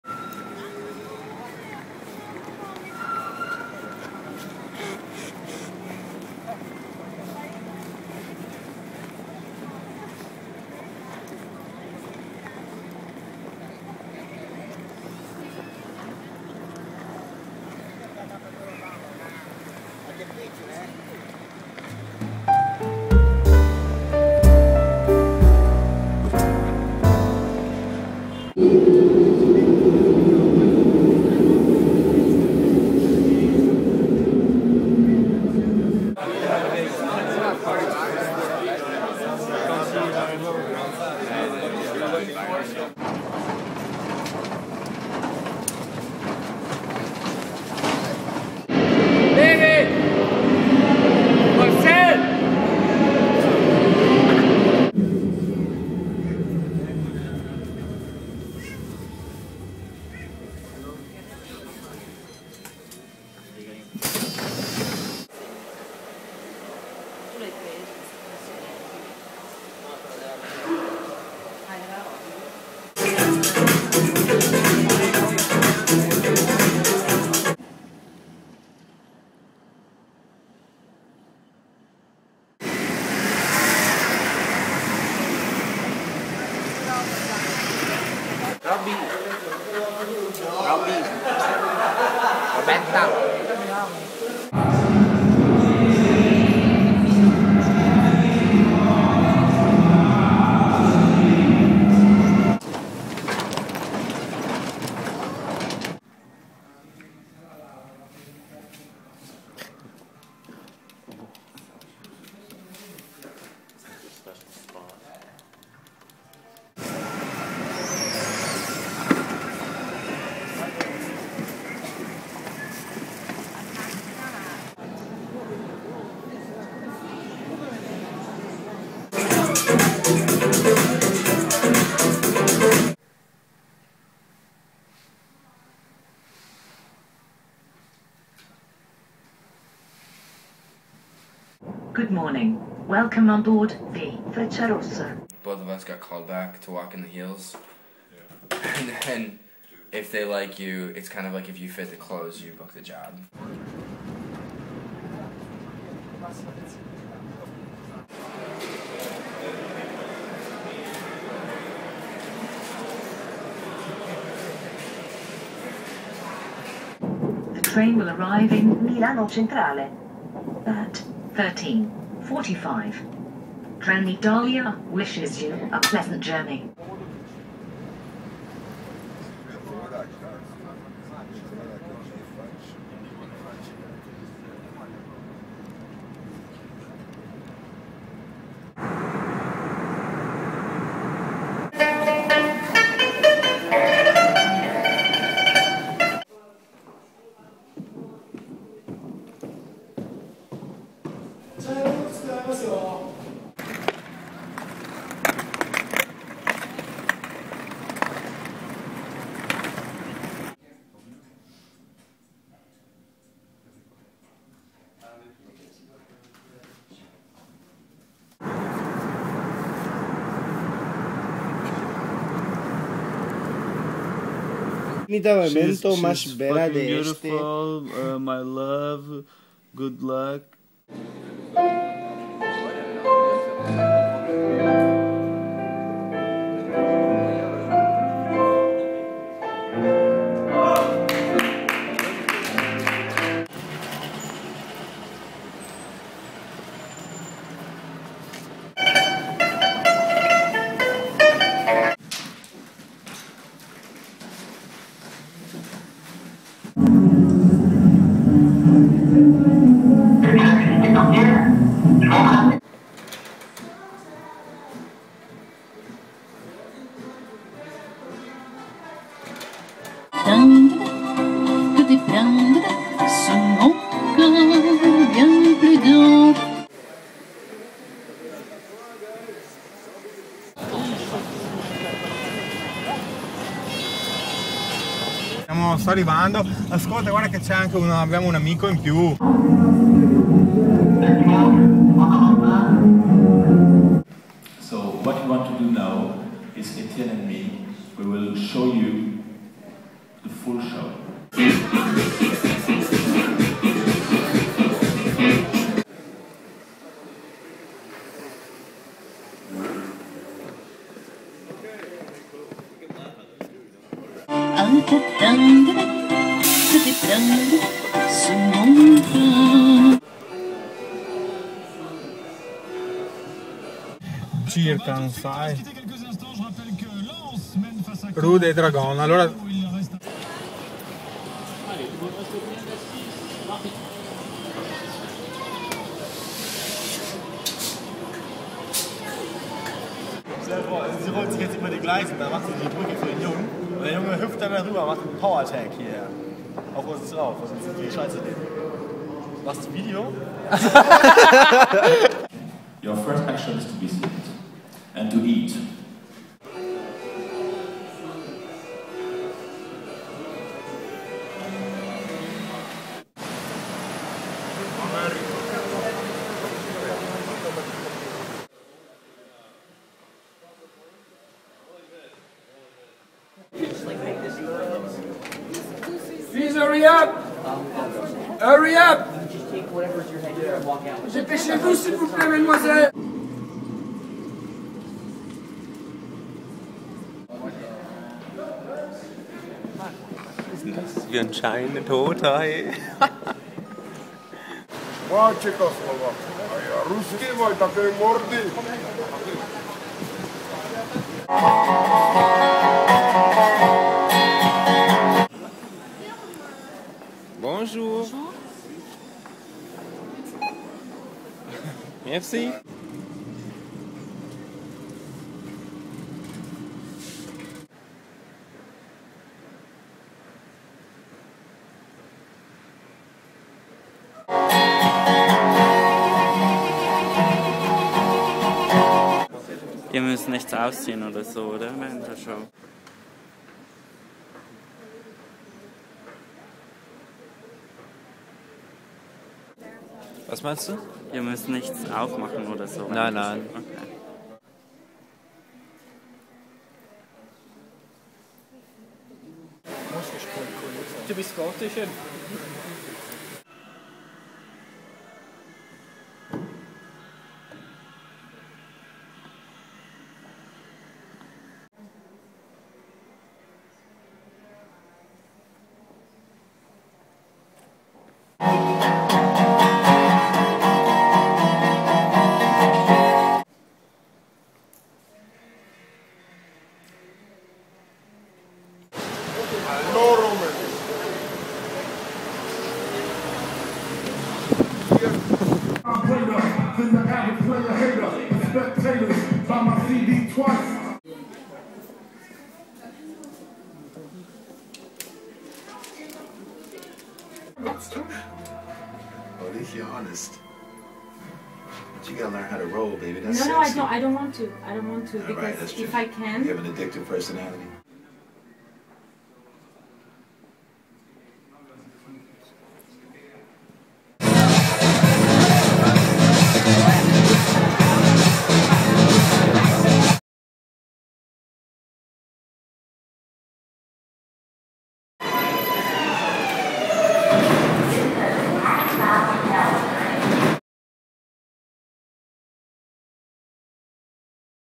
Berthold Rothas - My Fashion Week 2011-12 Part 1
Part 1 shows moments of my life during Fashion Week 2011-12 in Paris & Milan